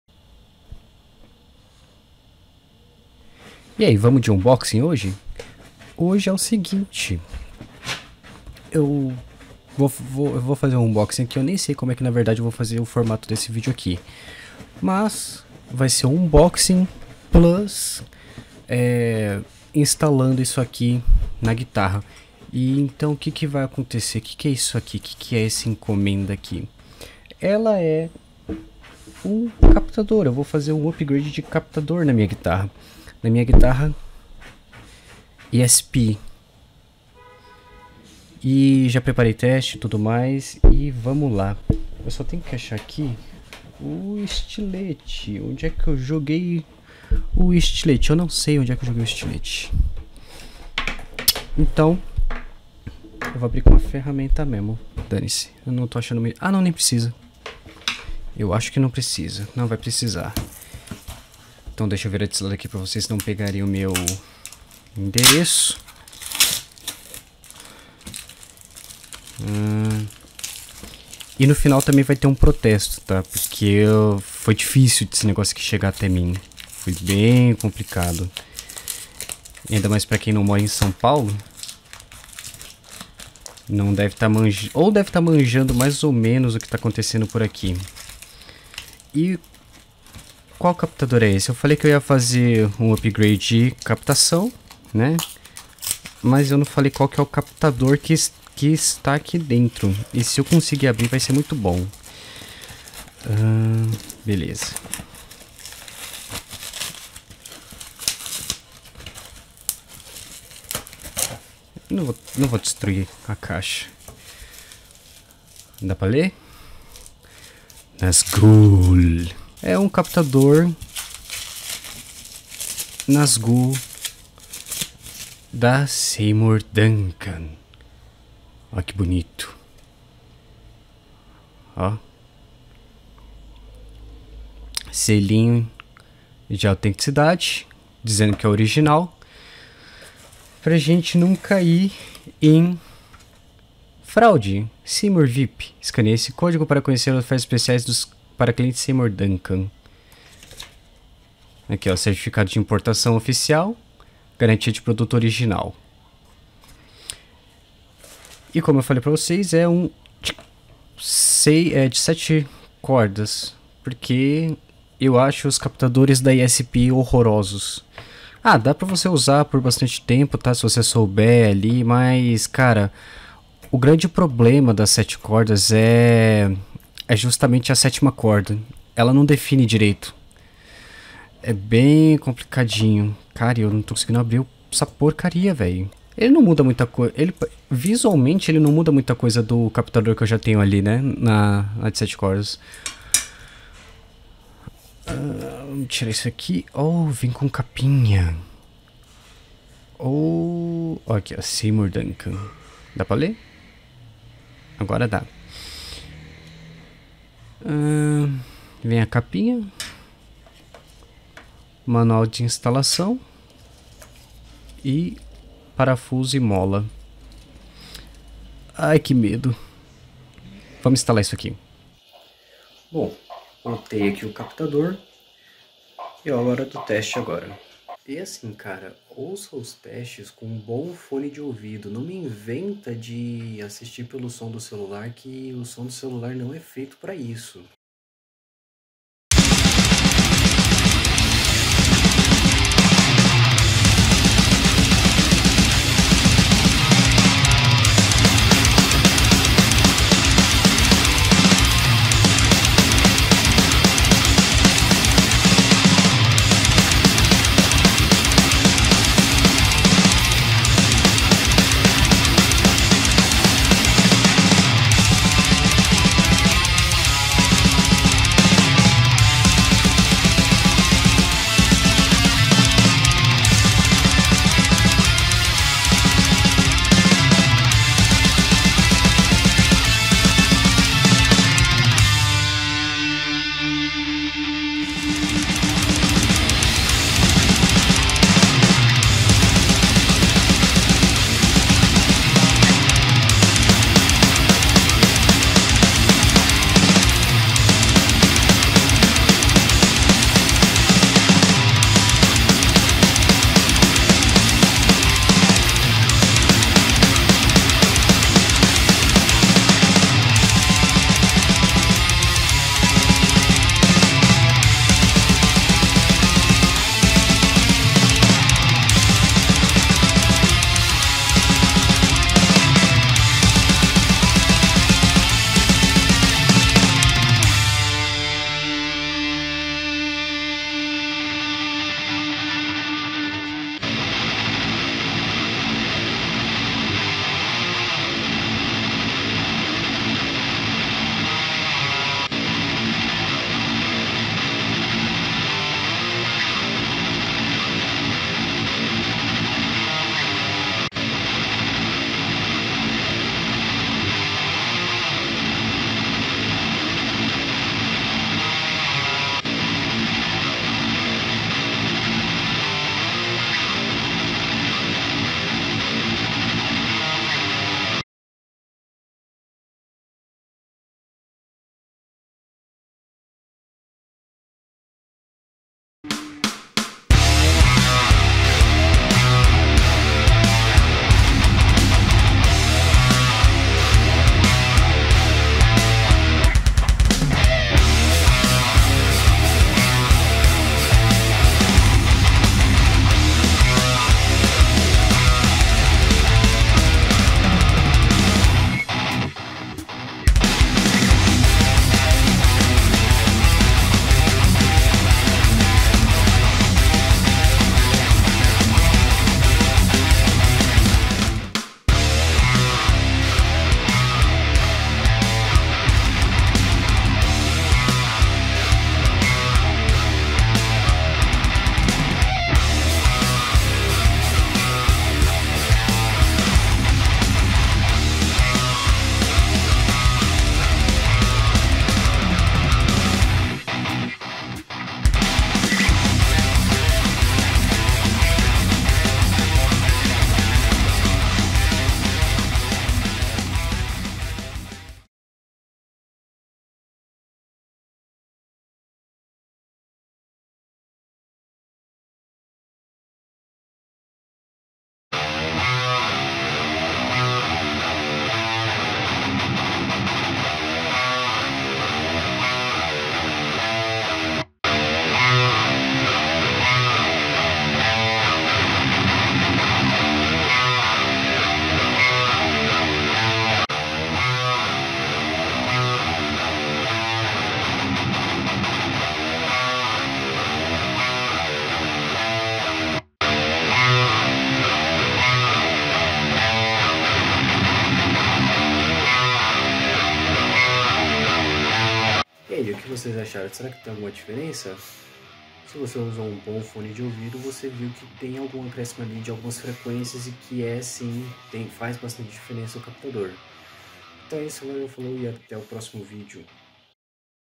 UNBOXING CAPTADOR NAZGÛL + TESTES
ESPecialmente hoje, venho com um unboxing e comparações entre captador de fabrica da ESP e o Nazgûl da Seymour Duncan!
Aviso: Ouça esse vídeo com um bom fone de ouvido, ou em algum sistema de som bacana, o alto-falante do celular não serve para esse tipo de coisa!!
English: ESPecially today, I come with an unboxing and comparisons between ESP's factory pickup and Seymour Duncan's Nazgûl!
Warning: Listen to this video with good headphones, or on a nice sound system, the cell phone speaker is not suitable for this kind of thing!!

Sigam-me no instagram @romaodossantos

Equipamento/Gear:
Guitar: ESP M-17
Pickup - Nazgûl by Seymour Duncan
Strings: D'addario XL EXL115 0.10
Daw: Reaper
Plugins - MudKiller Plugin, Amped - Roots, NadIR Impulse Response Loader,  Waves Packages

Blz, eu vou ficando por aqui, vlw por
terem assistido, flw e até uma próxima vez!!